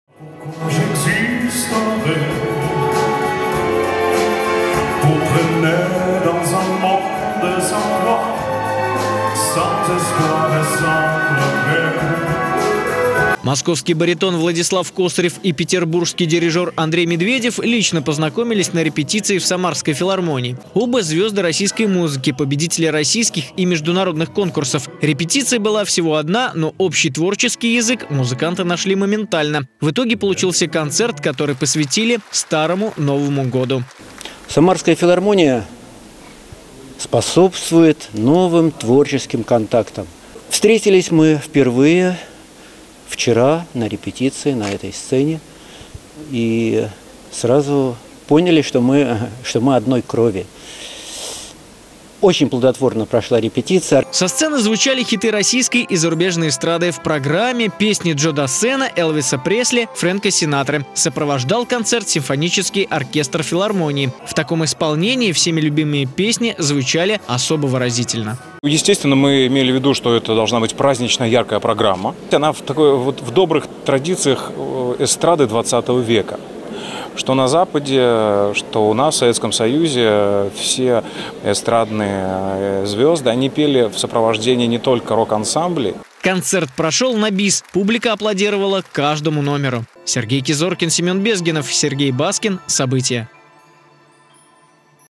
В филармонии состоялся праздничный симфонический концерт "И снова Старый Новый год"
Джо Дассен и Элвис Пресли в оркестровой аранжировке. В Самарской филармонии прошел концерт,  который посвятили Старому-Новому Году.  Солировал баритон Владислав Косырев, победитель международных конкурсов. Дирижировал известный петербурский аранжировщик и композитор Андрей Медведев. Хиты российской и зарубежной эстрады послушал и наш корресподент.